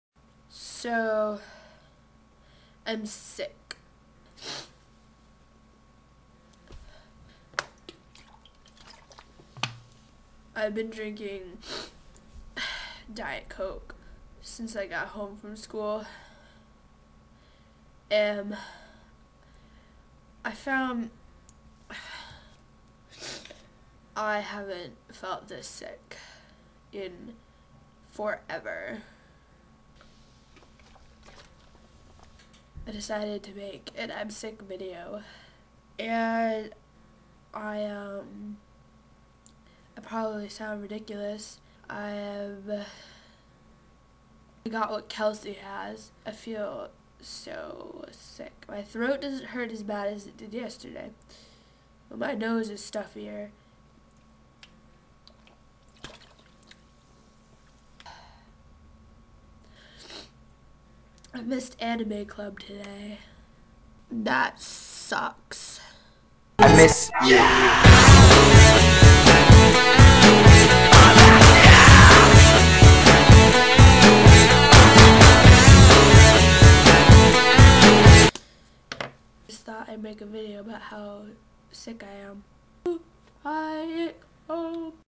I'm Sick
sick day. i could barely pronounce words correctly // "amb... i found..."